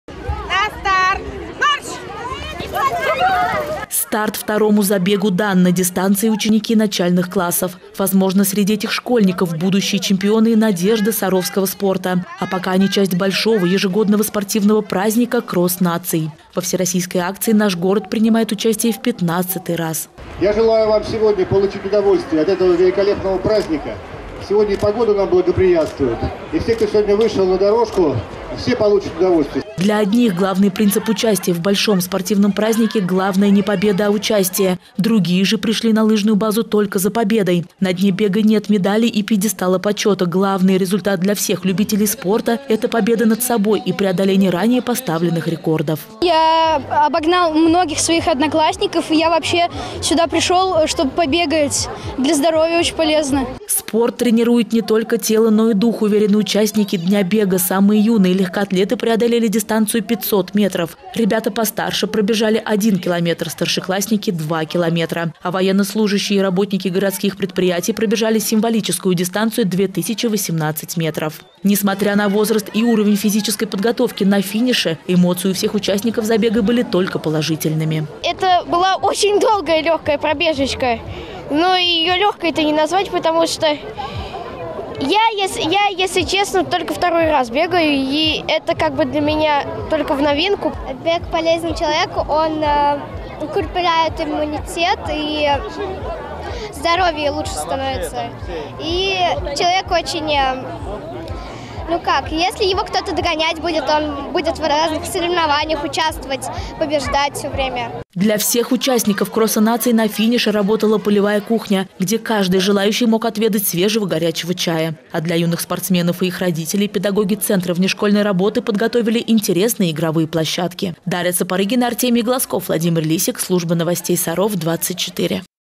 Кросс наций - 2018
Саровчане приняли участие в «Кроссе наций – 2018», который приурочили к Международному дню бега. На старт спортивного праздника вышли 1,5 млн россиян из 84 регионов нашей страны. В Сарове ко всероссийской акции присоединились более 3,5 тыс. горожан.